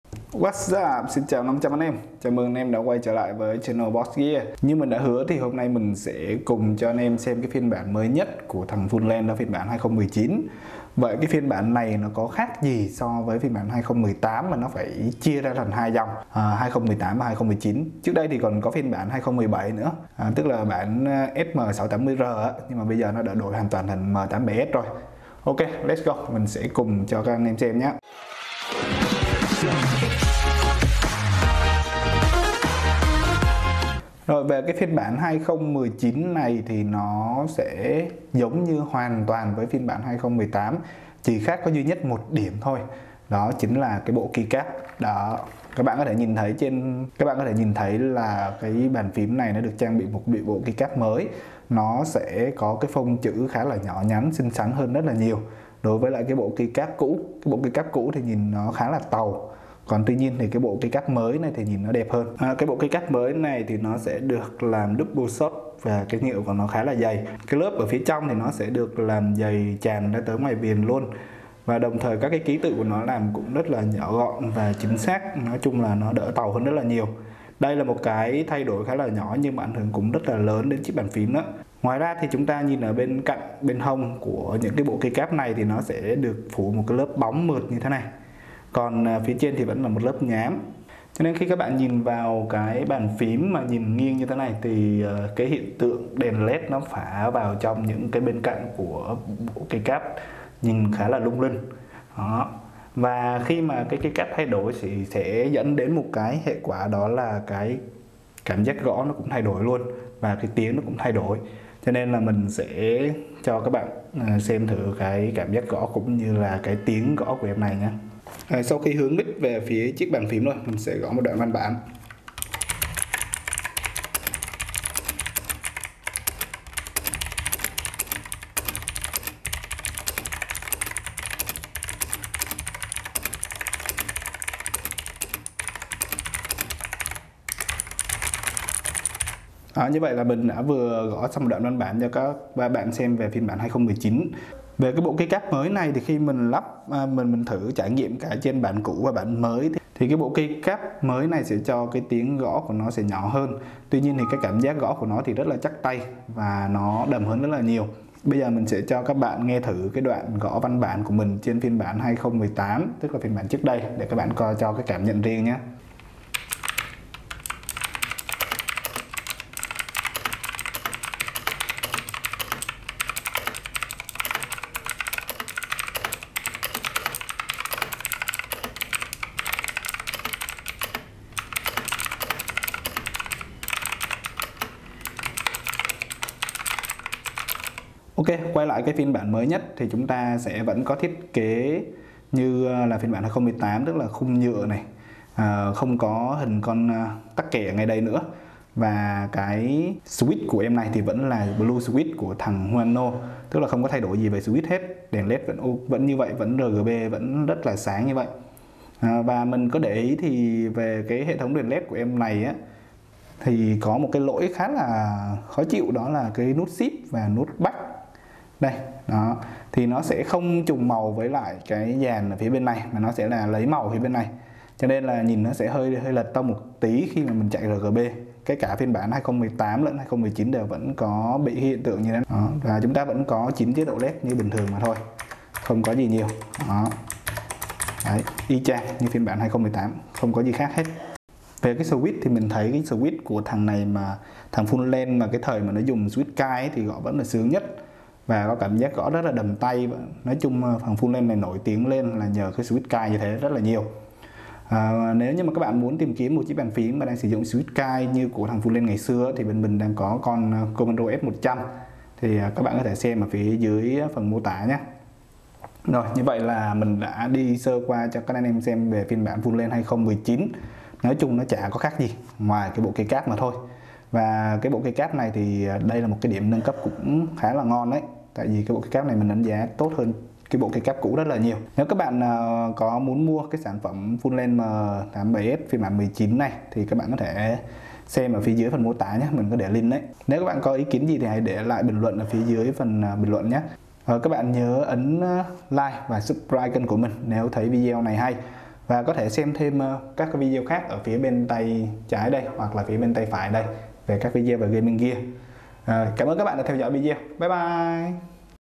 Bàn Phím Cơ Fuhlen M87S Phiên Bản 2019 Có Gì Khác?
Bàn Phím Cơ Fuhlen M87S RGB ( Phiên Bản 2019 – Bảo Hành 2 Năm )

Kích thước: 195×360 mm.
Chất liệu phím nhựa ABS Double-shot không tróc sơn theo thời gian.
Chế độ led: Led RGB .
​Cáp dài : 1,5m Bọc dù có cục chống nhiễu.
Phím cơ Blue SW. ( Huano )
Độ bền : 50 triệu lần nhấn.
Số phím: 87 phím.
Cân nặng: 1,2 kg.
Bảo hành chính hãng 2 năm.